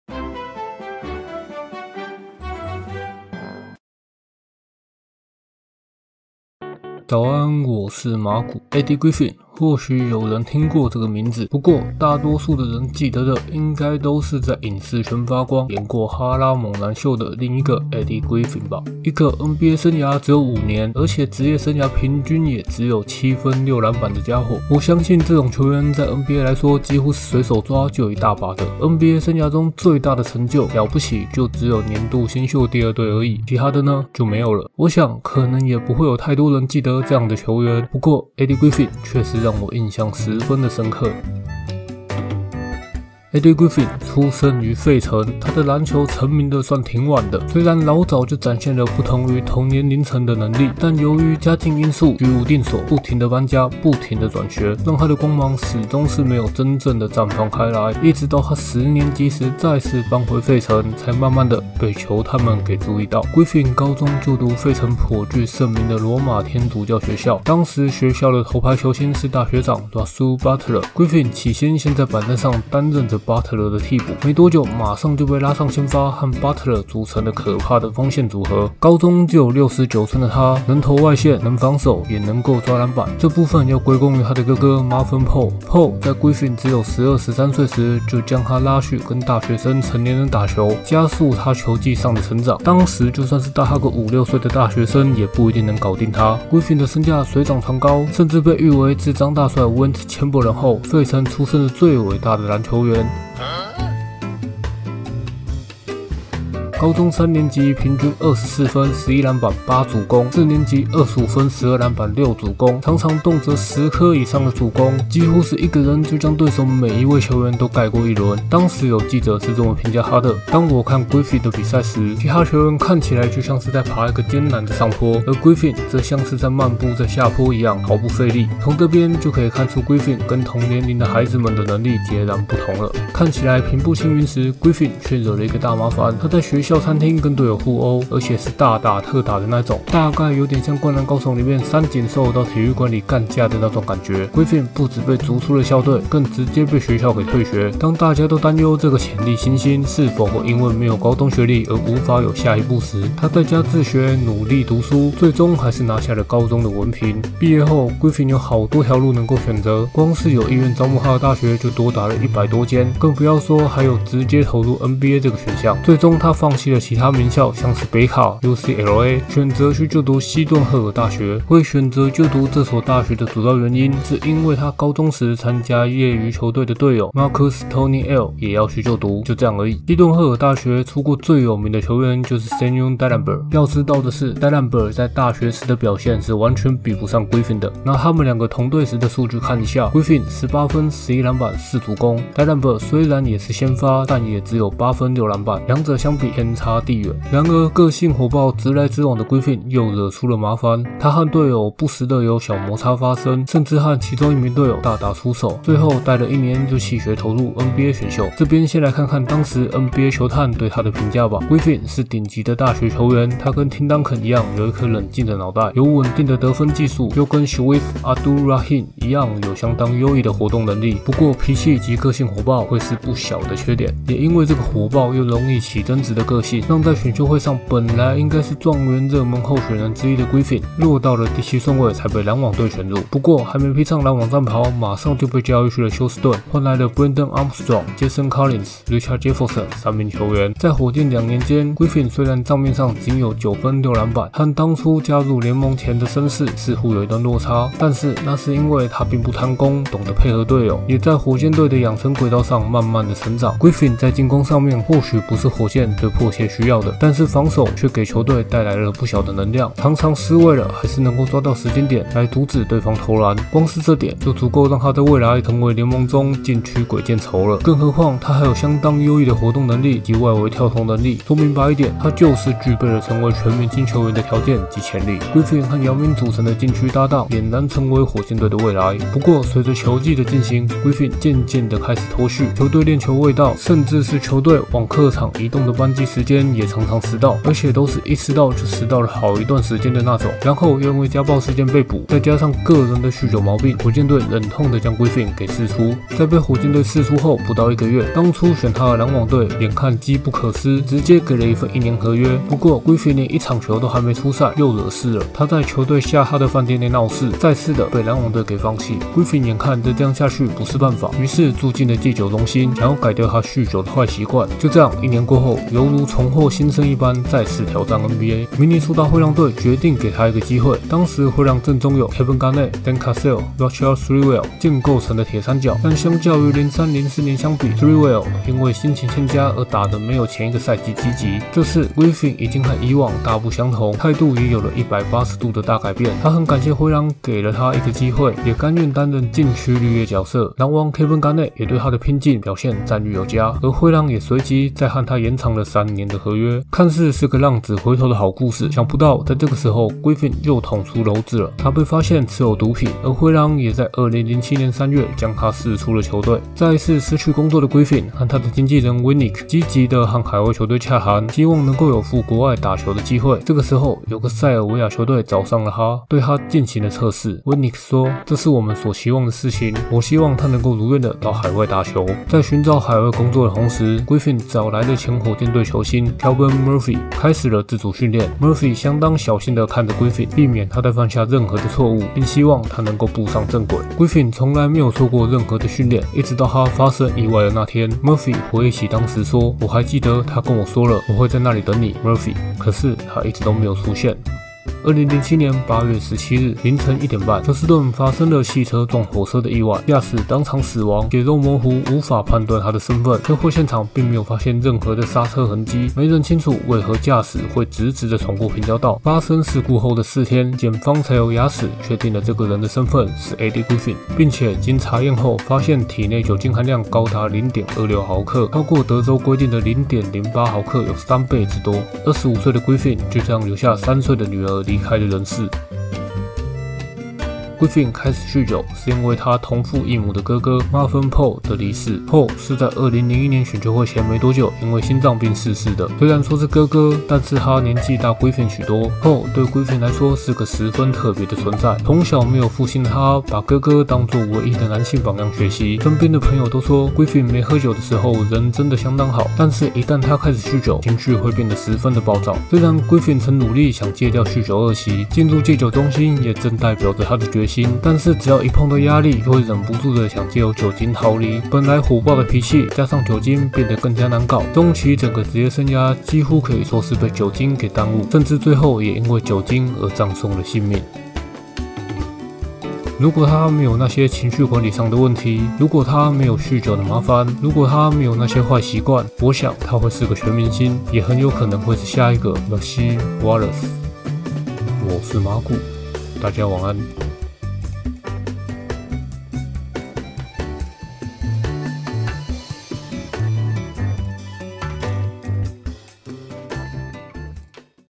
不曾來過的全明星－Eddie Griffin｜NBA球員故事｜早安馬古
有太多太多因為酗酒誤了自己前途的球員，Eddie Griffin更是因此而喪生…

0:00 Intro&Preface
0:42 兒時及求學階段
3:28 選秀及火箭隊時期
4:56 戒酒及灰狼時期
5:56 離開NBA
6:34 意外
7:53 Afterword